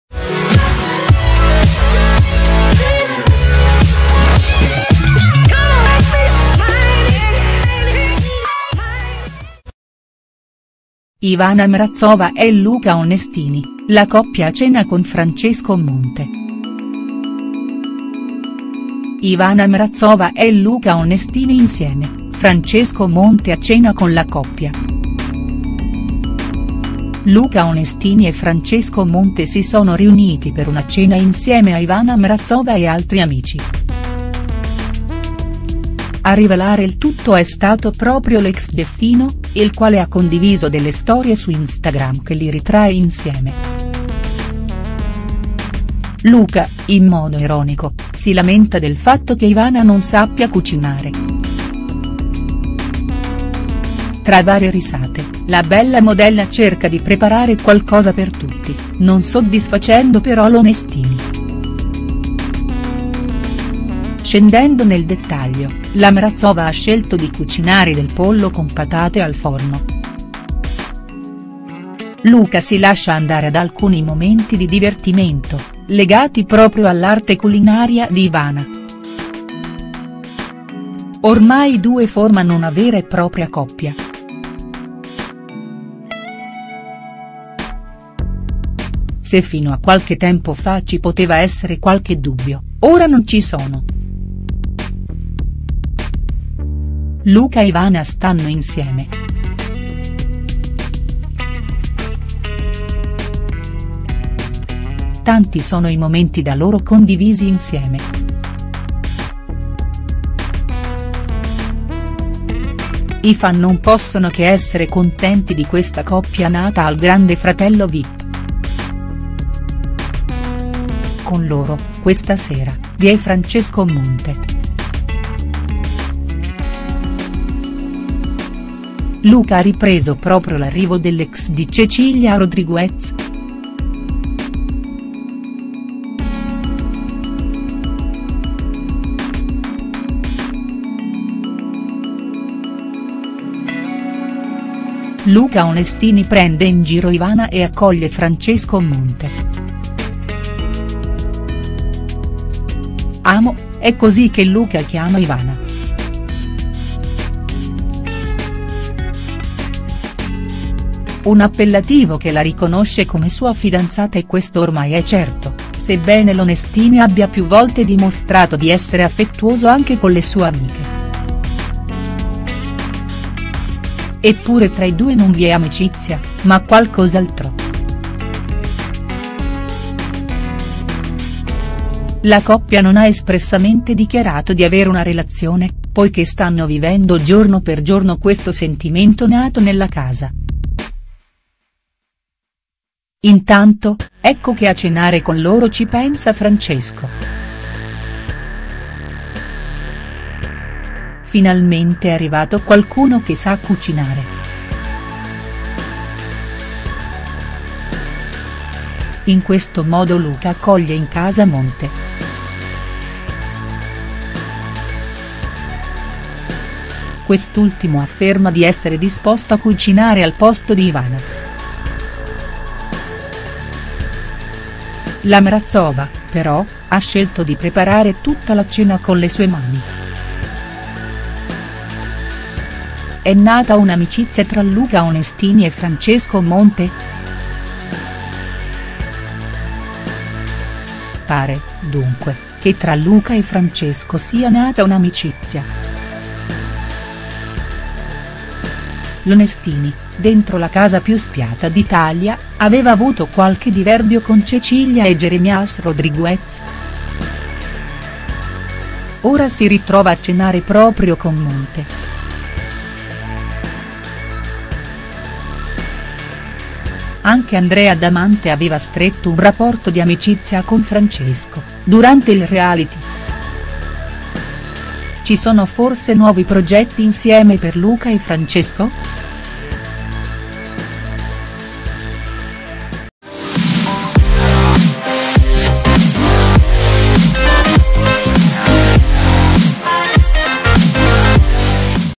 Ivana Mrazova e Luca Onestini: la coppia a cena con Francesco Monte| STARS NEWS
Ivana Mrazova e Luca Onestini si mostrano felici e spensierati mentre preparano la cena, a cui si aggiunge anche Francesco Monte